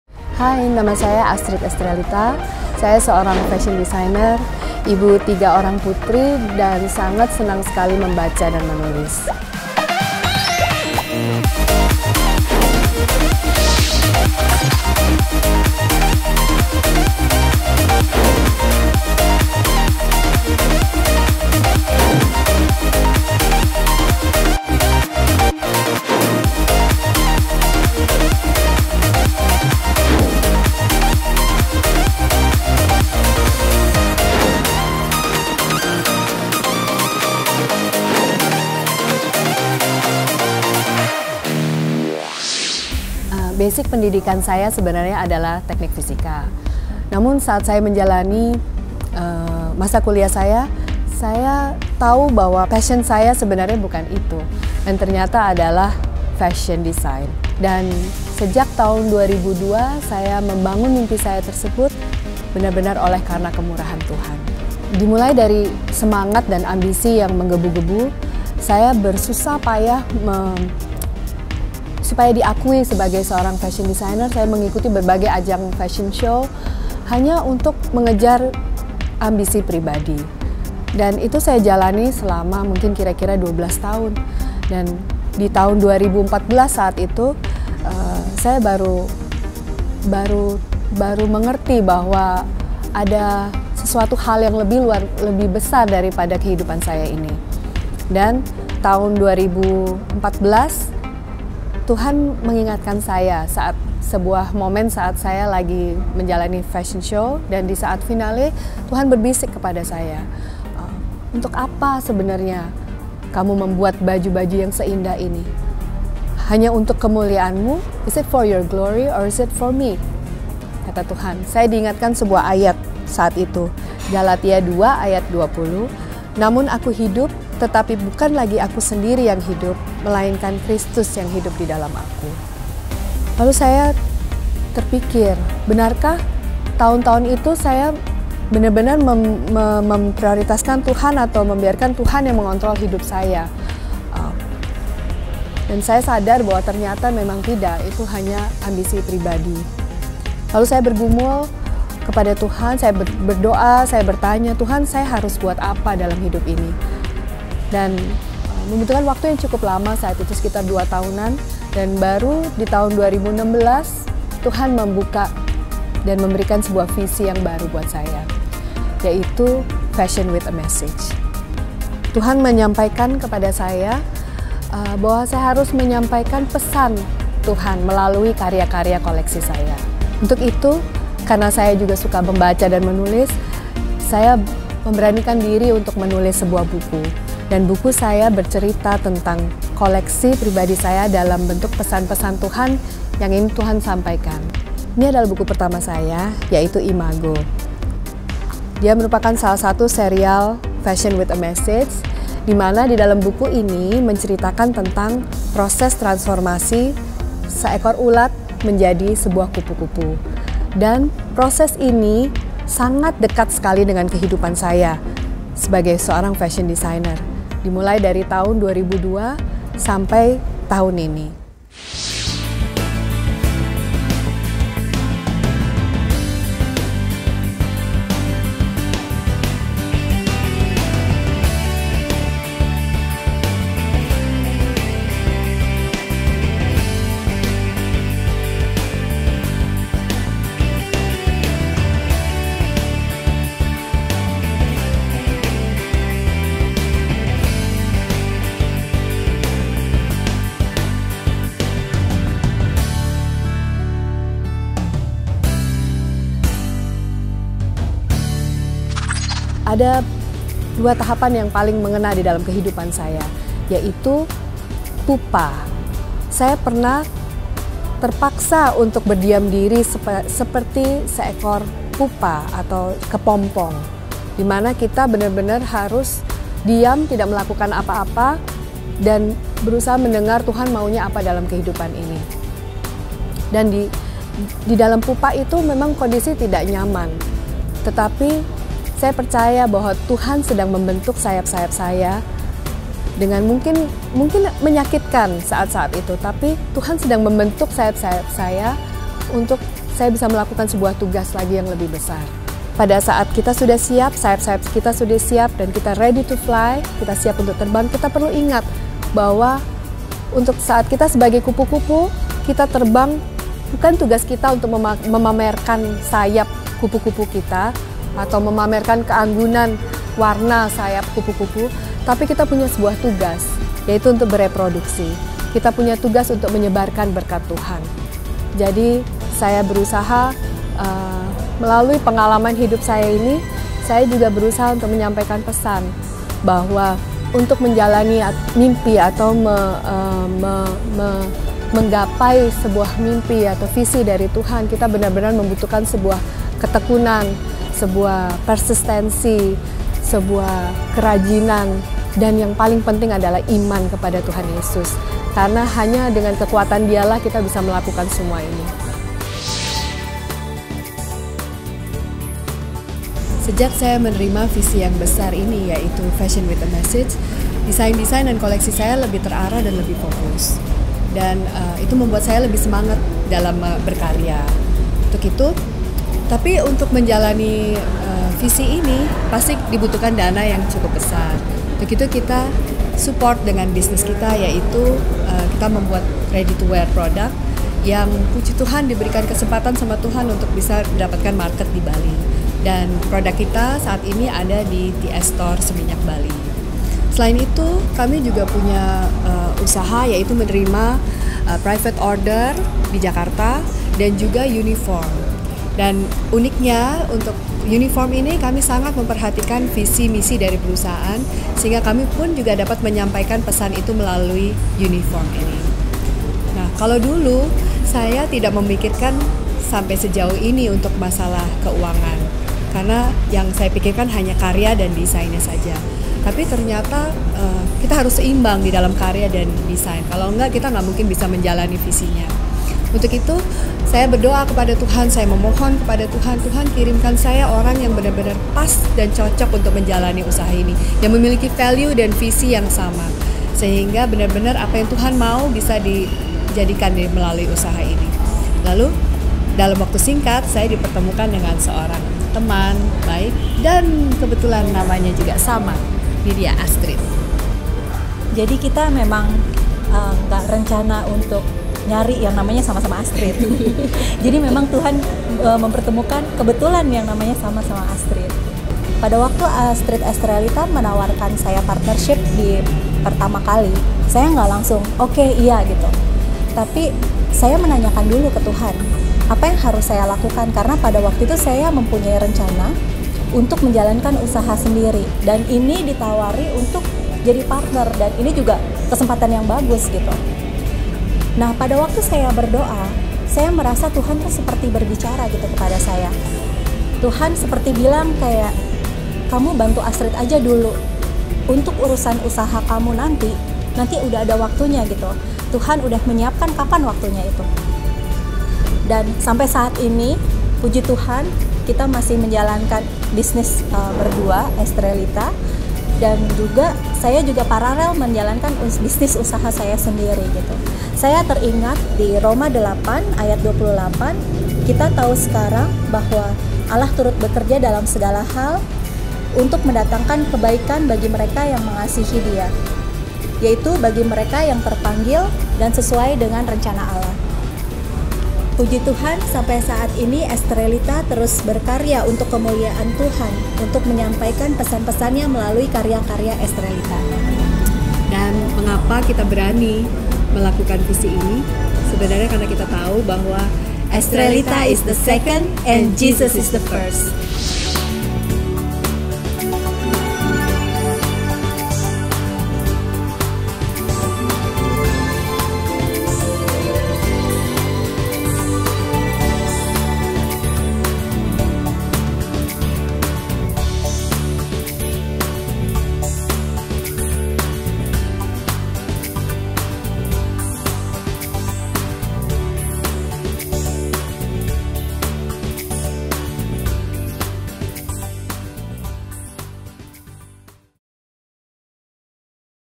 Ep.10 Astrid Estrelita
Astrid Estrelita

Desainer baju yang punya segudang bakat
Berani melangkah maju demi mimpi dan mengerjakan
semua hal dengan maksimal Menjalankan mimpinya
dengan tetap berpegang pada visi dari Tuhan

"Fashion with A Message"

Semua itu dijalaninya karena dia mengimani ayat ini:

Galatia 2:20
Namun aku hidup, tetapi bukan lagi aku sendiri
yang hidup, melainkan Kristus yang hidup di dalam aku.
Dan hidupku yang kuhidupi sekarang di dalam daging,
adalah hidup oleh iman dalam Anak Allah yang telah
mengasihi aku dan menyerahkan diri-Nya untuk aku.